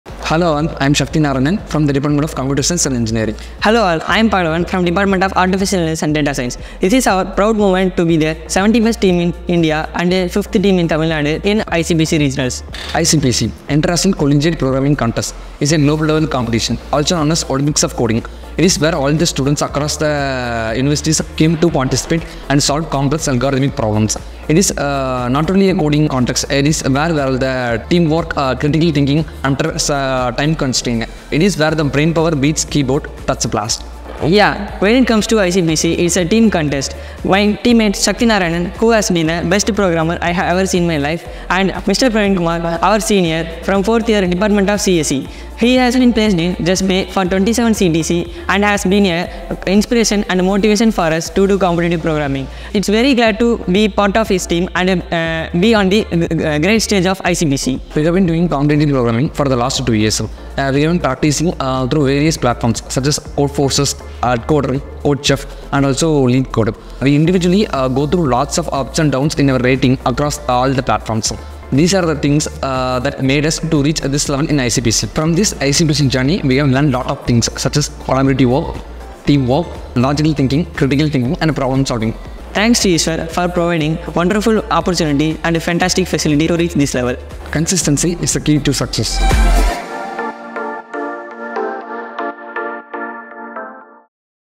Eshwarites have 👍triumphed in the World Largest and Prestigious 🖥️Coding Contest, ICPC
🌟 “Unleashing brilliance in every line of code, Eshwarites shine in the global🌎 coding arena” 🚀

Second time in a row, our Eshwarites have 👍triumphed in the World Largest and Prestigious 🖥️Coding Contest, ICPC - International Collegiate Programming Contest Amrithapuri Regionals – Dec 2023. 🏆

Kudos🙌 to our coding champions! 🥳

Huge applause to 🏆Pravinkumar S - CSE, 🏆Sakthi Narayanan R -CSE and 🏆Pagalavan Manoharan - AI & DS for securing an impressive 71st position among 228 teams in which 150 + teams are from IITs, IIITs, NITs, and Top Universities and 5th position among colleges from Tamilnadu - A true testament to their coding skills. 👏

Eshwarites – The Coding Maestros have etched their names in the coding hall of fame – Defining Excellence in Every Line. Congratulations Eshwarites for turning lines of code into a symphony of success. 🎉

Champions in Code, Icons in Intelligence!
Keep Coding, Keep Inspiring! 💻✨